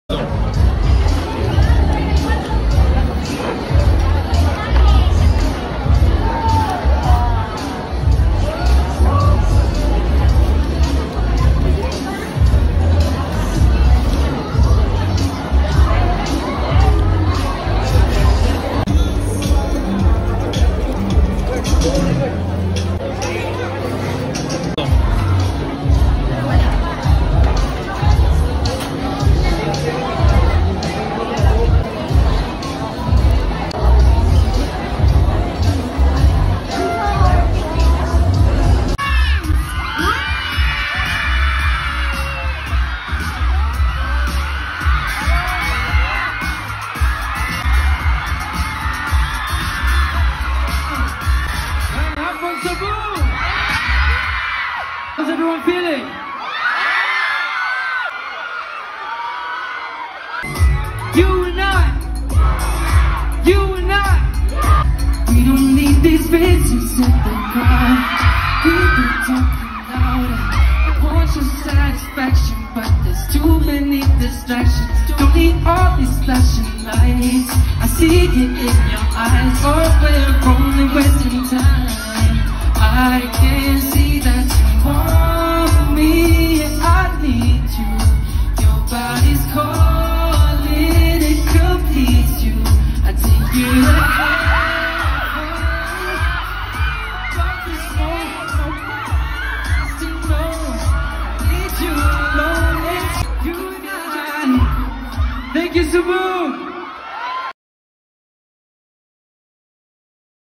never thought nga makakita ko ni James Reid! ka gwapo 🥹😍
kaon sa ta usa mutan aw ni James. Basin makuyapan nya ta kita ni James Reid haha 😅